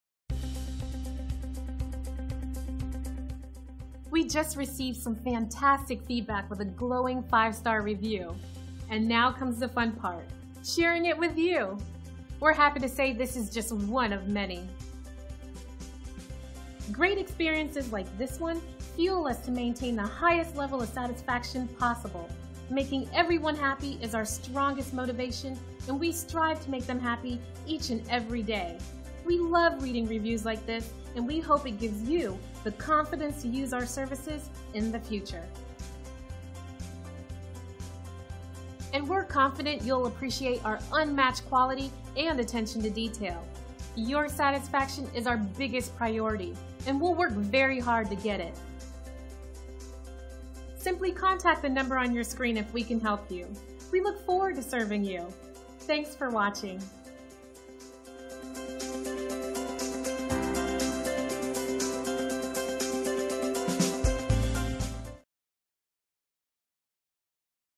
Five Star Review - Memphis, TN Periodontist - Periodontal Associates of Memphis
Memphis, TN Periodontist - 5 Star Review
Dr. Godat and his entire staff are very kind and caring people, and are sensitive to the comfort and well being of their patients. They are prompt in keeping appointment times, and are quite knowledgeable and professional in completing all work in a timely manner.

Periodontal Associates of Memphis
6268 Poplar Ave
Memphis, TN
38119